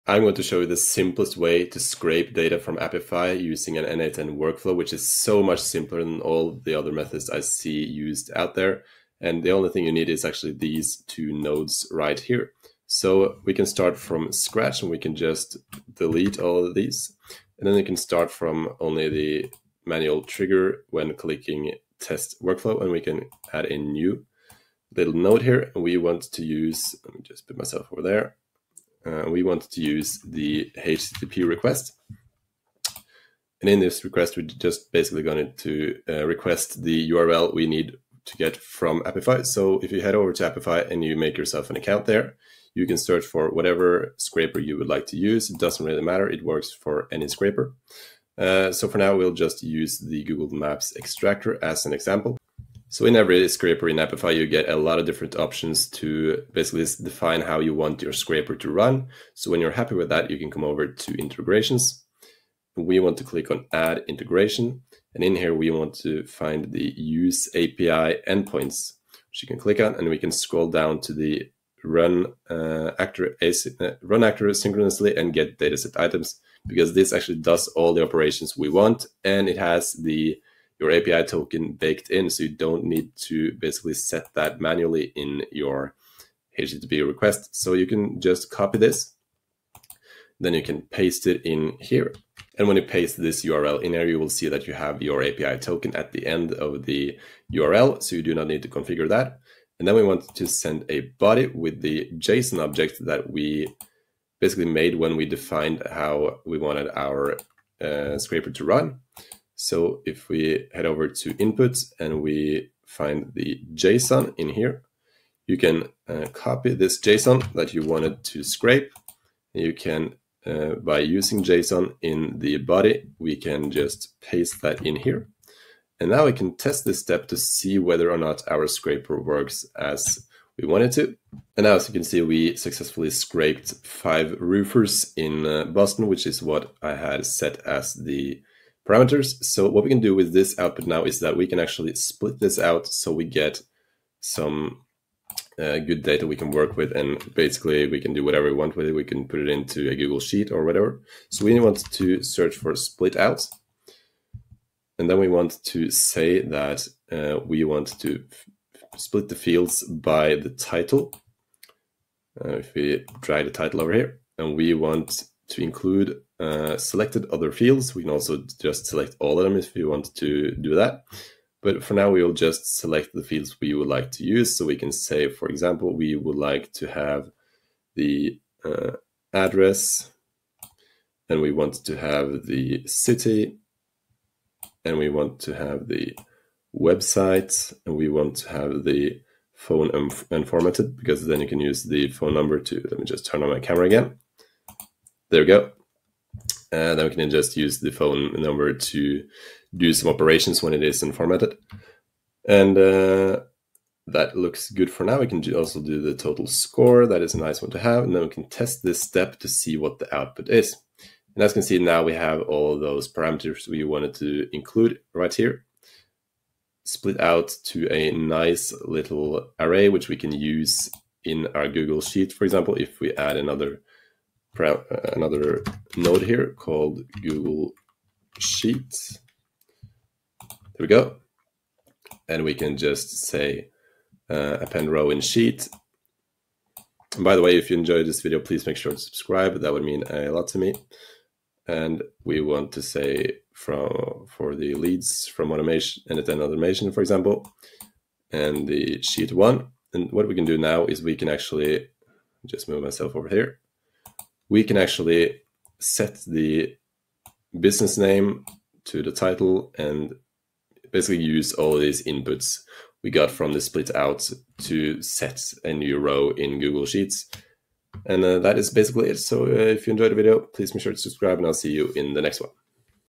How To Use Apify in N8N (BEST WAY)
Unlock the power of N8N automations and AI automations with this step-by-step guide on how to use Apify in N8N! In this video, we’ll explore the best way to integrate Apify with N8N, helping you supercharge your workflows and streamline your no-code automation processes. Whether you're scraping data from websites or building robust automation solutions, this tutorial will give you the tools and knowledge to succeed.

Discover how to:

Set up Apify with N8N for seamless data scraping and processing.
Create custom workflows without writing a single line of code.
Optimize your automation pipeline using no-code tools.
This video is perfect for:

Beginners exploring no-code automation tools.
Advanced users looking to enhance their N8N automation workflows.
Developers and enthusiasts in the automation and AI automation space.
Stay ahead in the world of no-code with our actionable tips and hands-on demonstrations!